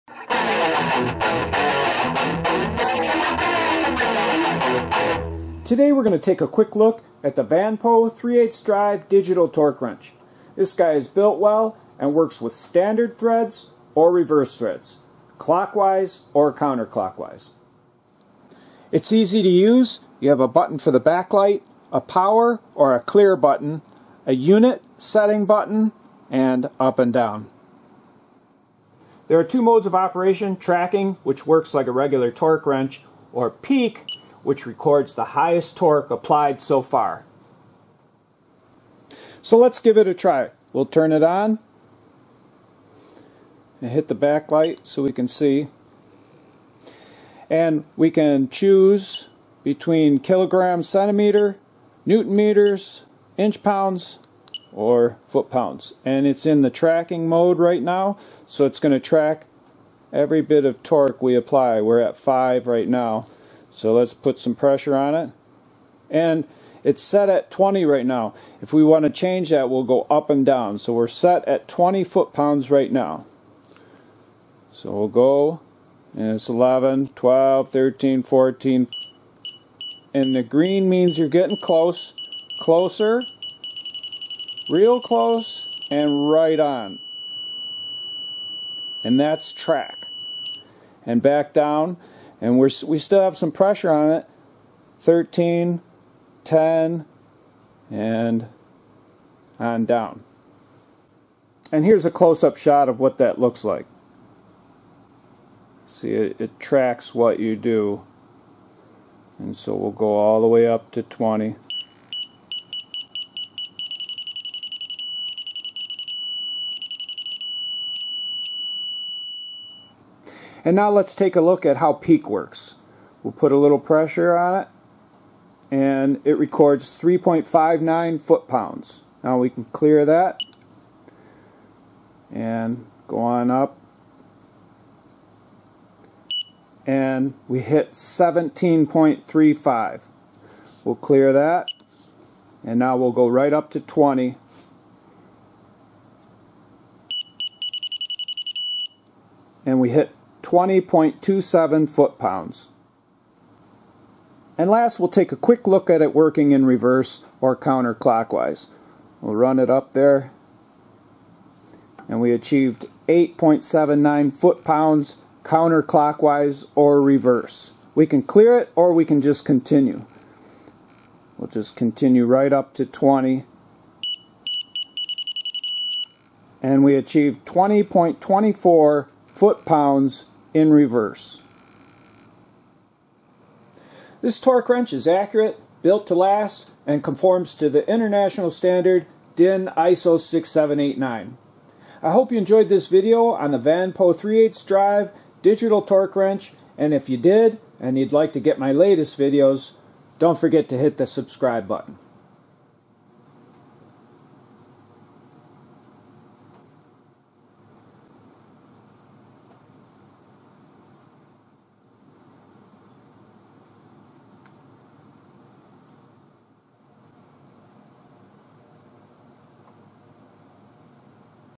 VANPO Digital Torque Wrench with Peak and Track Mode
A Review of the VANPO digital torque wrench. Features an advanced LCD display, provides kg-cm, Nm, in-lb & ft-lb units, quality construction and a back light.  Product link below -